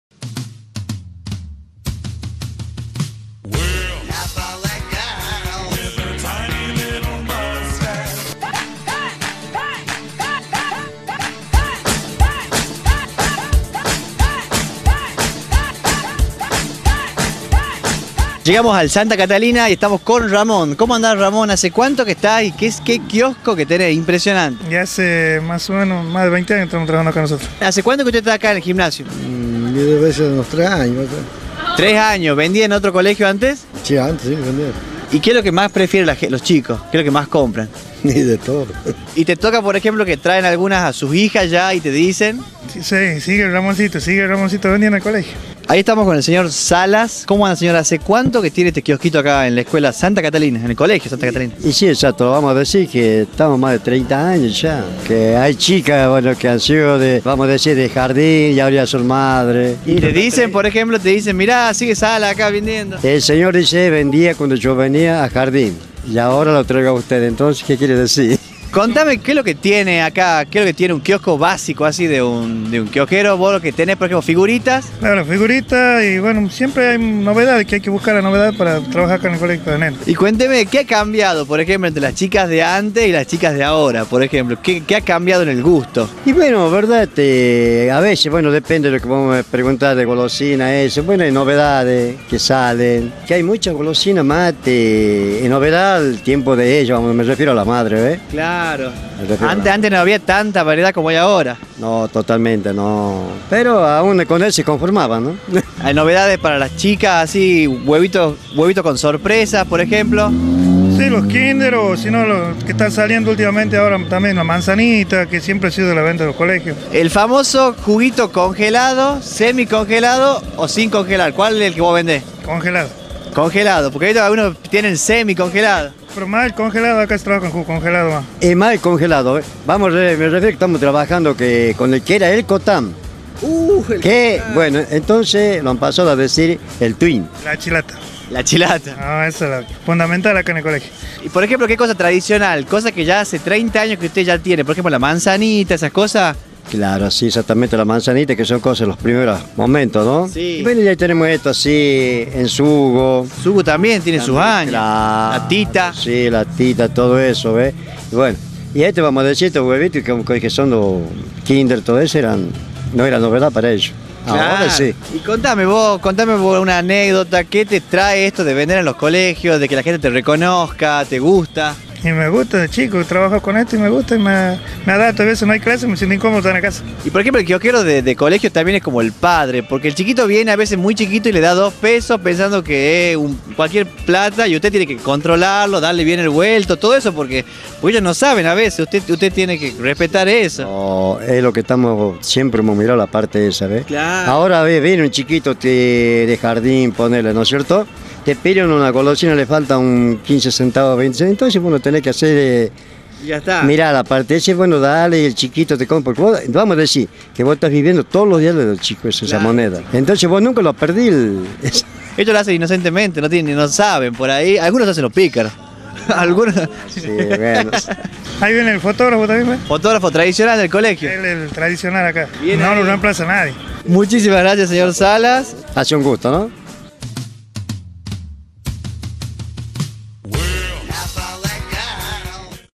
Kioskeros de Colegios en la Republica de Tucuman 1ª Parte
Los tipos que venden golosinas en los colegios estan ahi ahce muchos años conoce un poco mas de su historia